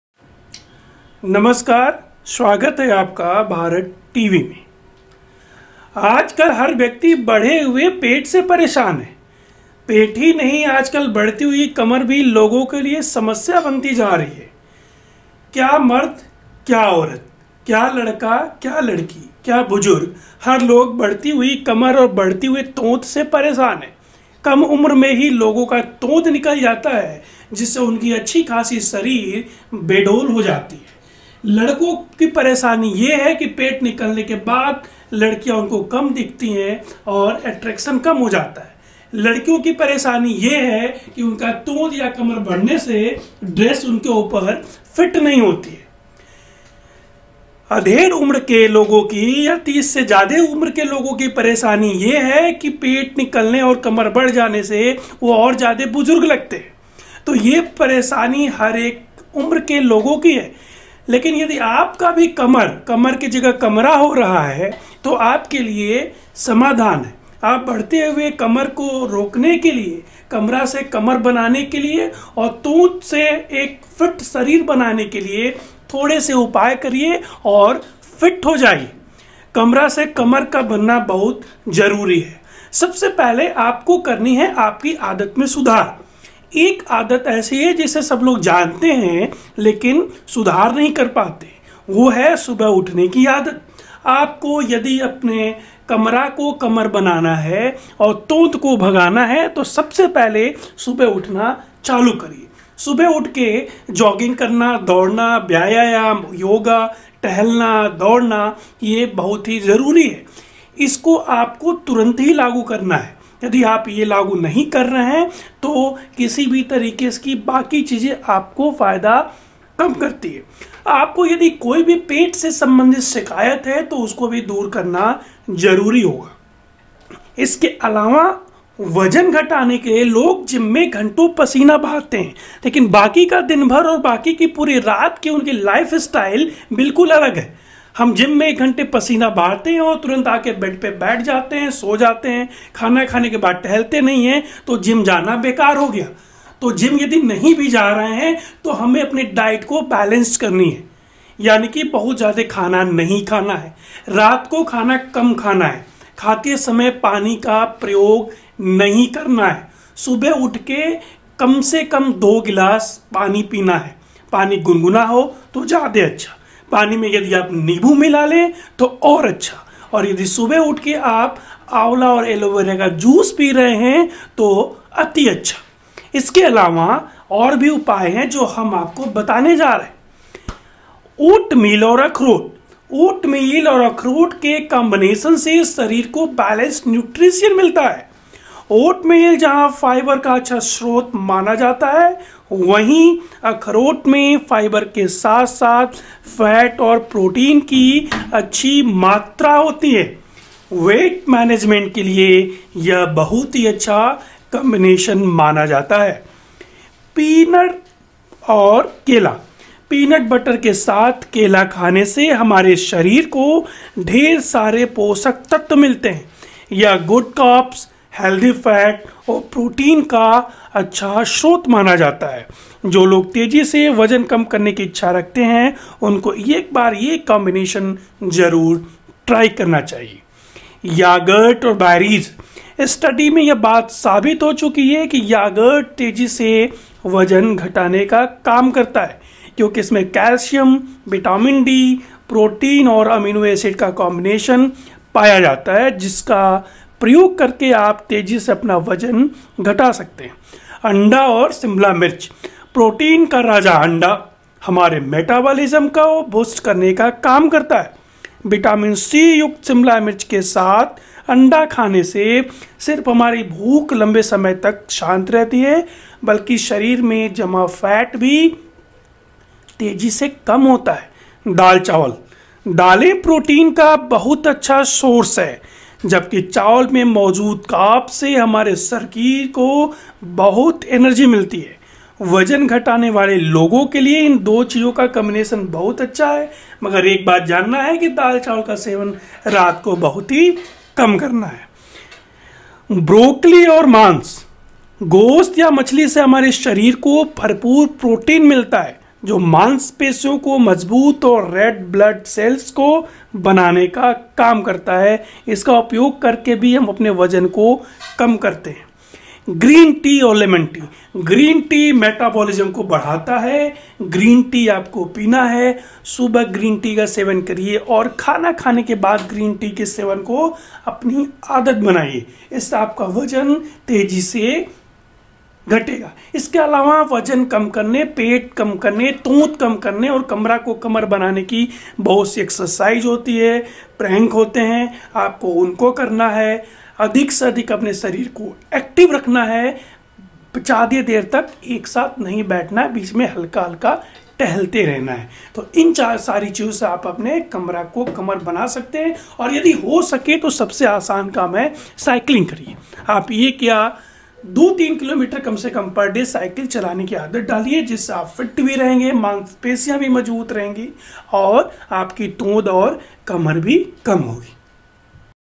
पतली कमर चाहिए तो करें ये वर्कआउट और खाए ये चीज How to loose Belly
पतली कमर चाहिए तो करें ये वर्कआउट और खाए ये चीज
Yadi aap bhi badhate pet ya kamar ya tond se pareshan hain to aalo ham batayenge ki kamar kaise kam kare. is video me pet kam karne ke upay aur kamar ko patla karne ke upay ke bare me batayenge.